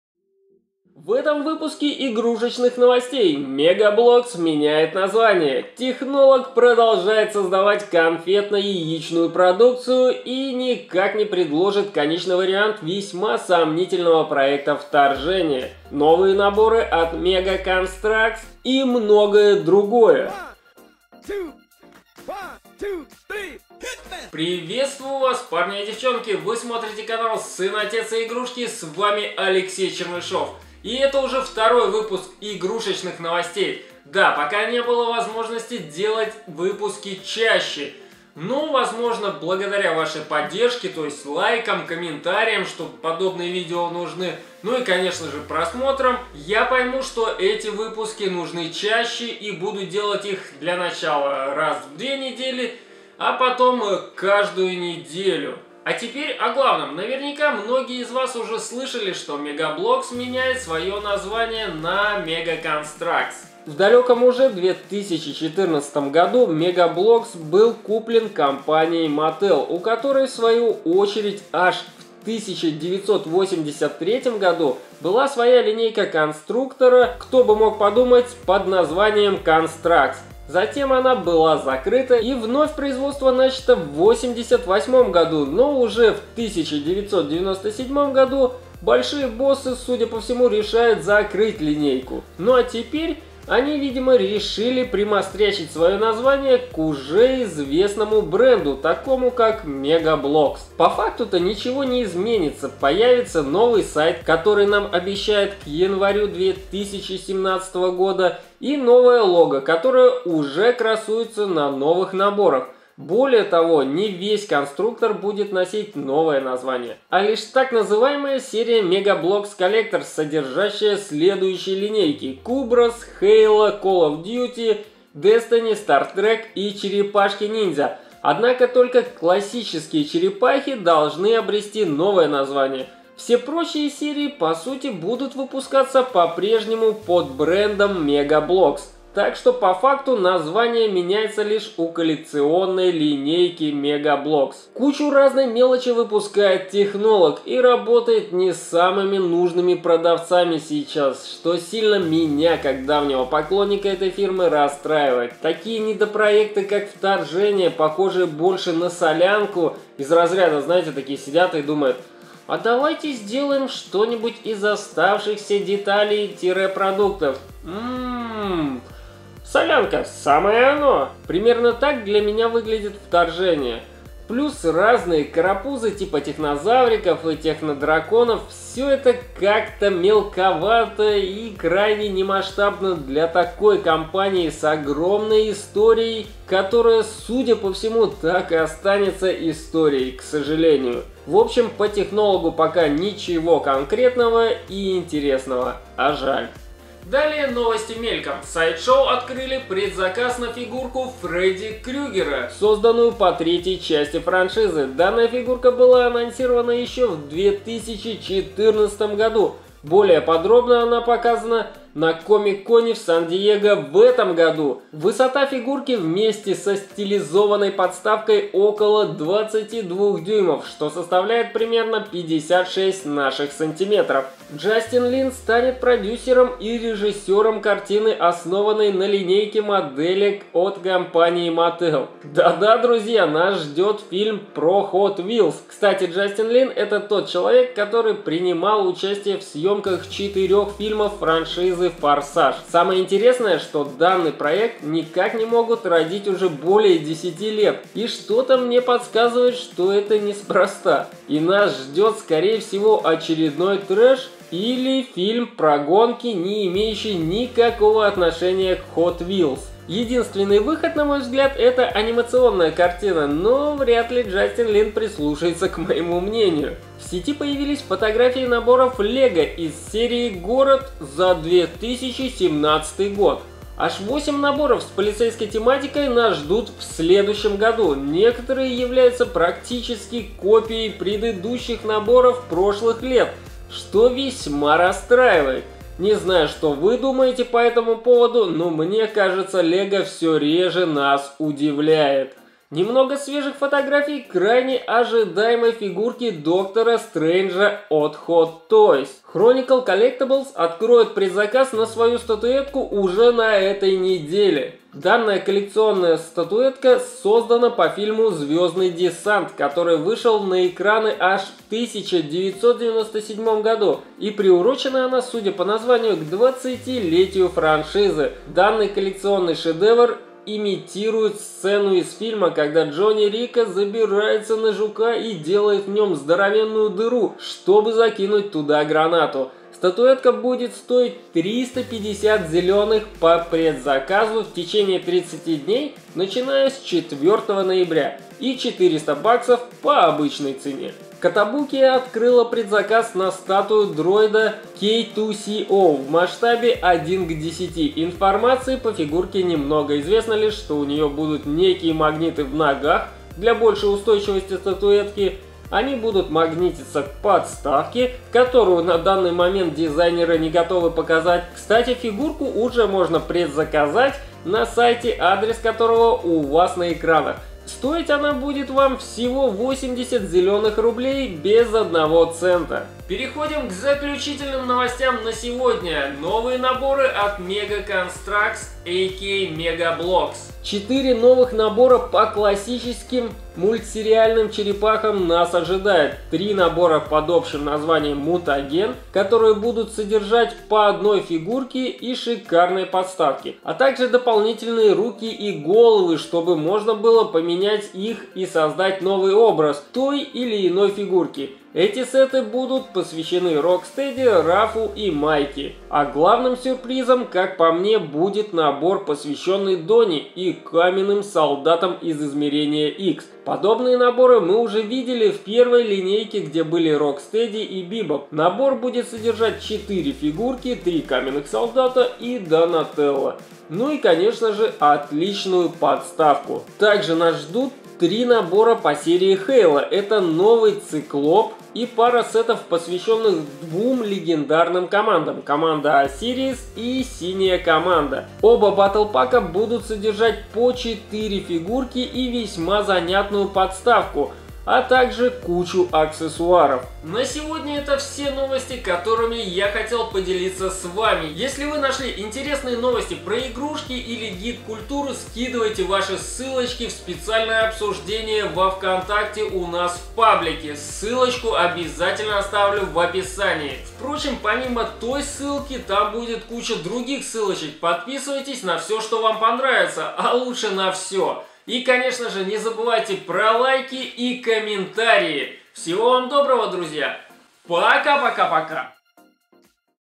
Игрушечные Новости | Mega Bloks меняет название на Mega Construx, наборы LEGO City 2017
Приветствую вас парни и девчонки! Сегодня я расскажу вам немного новостей  игрушечной индустрии и гик культуры. В этом выпуске вы узнаете о том, что:

- Мега Блокс (Mega Bloks) меняет название на Мега Констракс (Mega Construx)
- Немного новостей про Технолог
- Открыт предзаказ на фигурку Фредди Крюгера (A Nightmare On Elm Street 3: Dream Warriors – Freddy Krueger Premium Format Figure) от компании Sideshow.
- Джастин Лин (Justin Lin) снимет фильм про Hot Wheels
- Фотографии новых наборов LEGO City за 2017 год
- Свежие фотографии фигурки Доктора Стрэнджа от Хот Тойс (Hot Toys Dr. Strange )
- Открыт предзаказ на статуэтку (Starship Trooper Tanker Bug 20th Anniversary Statue) от Chronicle Collectibles по фильму Звездный Десант.
- Новые наборы от Mega Construx - четыре набора по Классическим Черепахам (Teenage Mutant Ninja Turtles - Michelangelo Mutagen, Rockstedy Mutagen, Raphael Mutagen), а также батл-пак - Donatello vs Rock Soldiers. Более того еще и три набор от Mega Construx по серии HALO - Cyclop, Team Osiris и Team Blue.